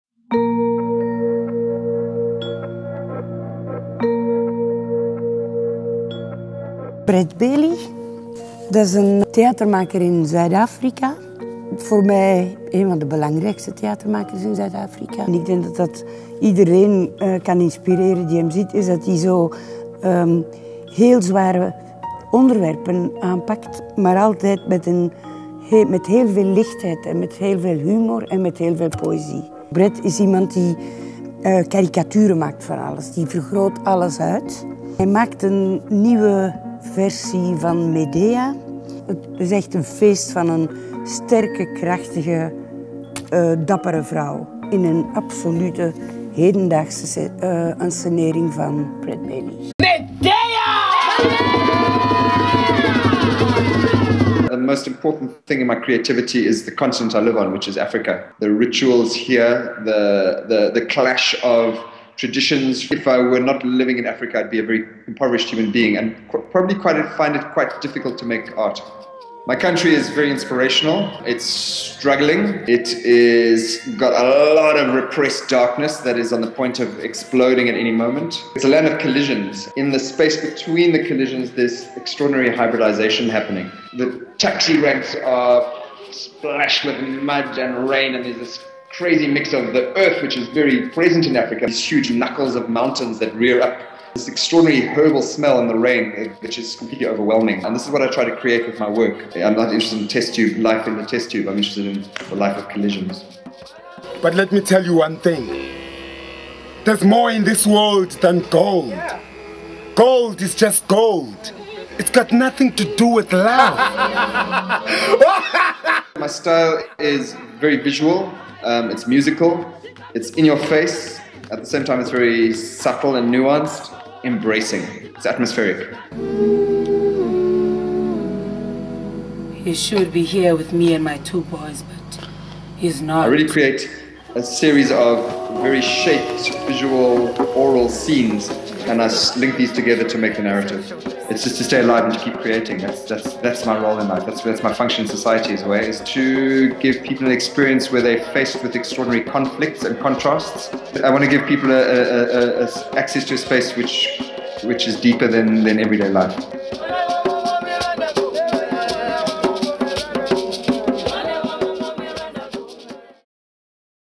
Brett Bailey over zijn voorstelling MedEia
Frie Leysen over Brett Bailey en Bailey over Zuid-Afrika en zijn voorstelling MedEia.